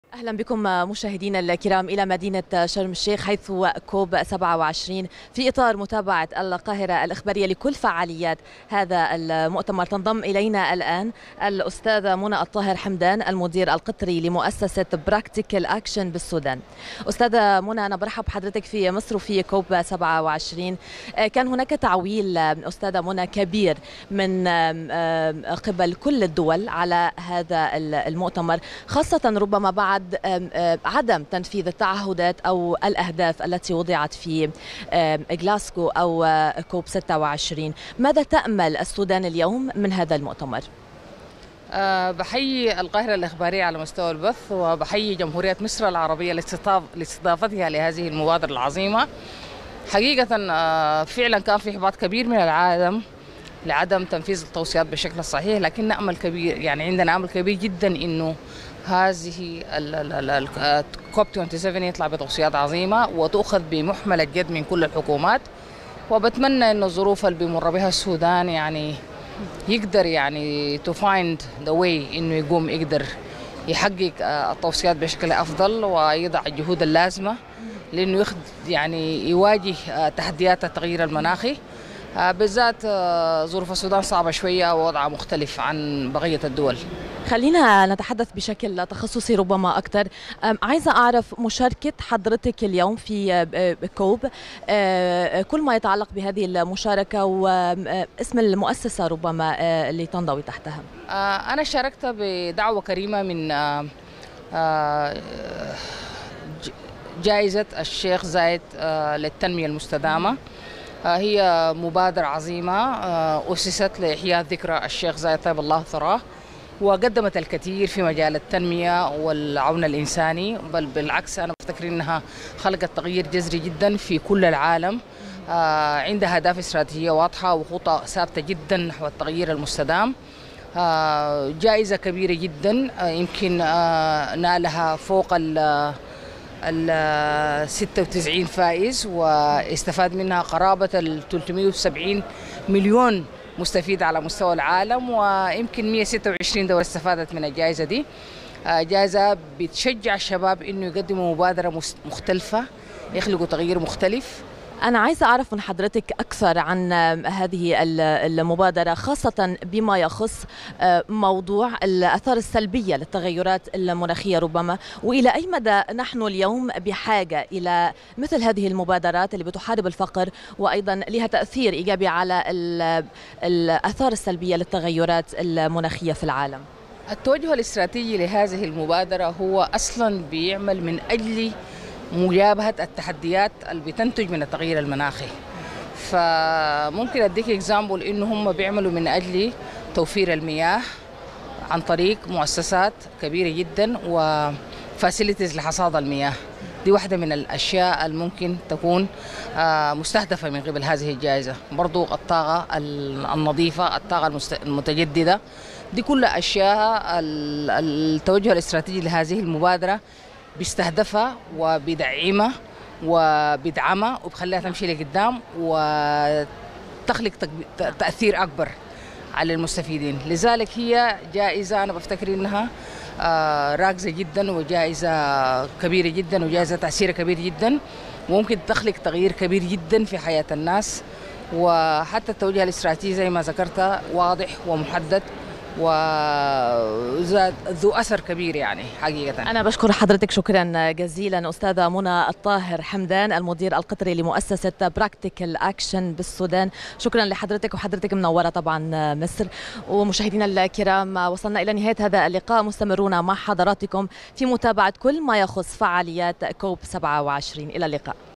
مدير مؤسسة براكتيكال أكشن: إحباطات من عدم تنفيذ التوصيات السابقة للمناخ ونأمل بأن يكون كوب٢٧ مختلفا
منى الطاهر  مدير مؤسسة براكتكال أكشن بالسودان: هناك احباطات من عدم تنفيذ التوصيات السابقة للمناخ ونأمل بأن يكون كوب٢٧ مختلف ويتم تنفيذ القرارت التي تؤخذ

٨-١١-٢٠٢٢

هنا_القاهرة_عاصمة_الخبر 8/11/2022